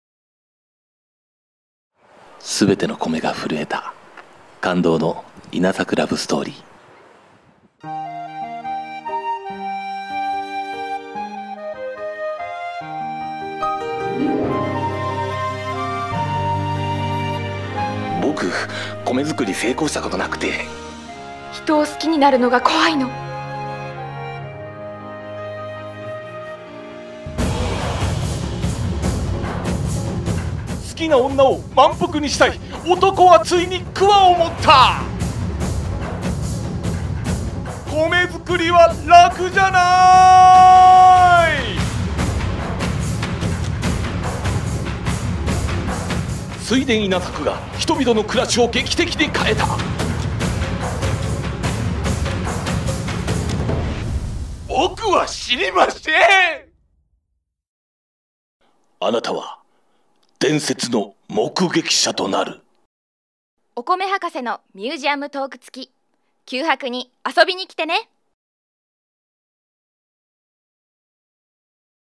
九博オリジナル劇「弥生人現る!!～101回目の米作り」予告編
文化交流展関連イベント「弥生人現る!!～101回目の米作り」
弥生時代の米作りのすべてがわかる、九博オリジナル劇・予告編！
公演日時：第1回　8月10日（金）・第2回　8月18日（土）

【出演】作郎：宮木秀明　弥生：倉本星華　米助：小澤佳憲
【協力】国史跡板付遺跡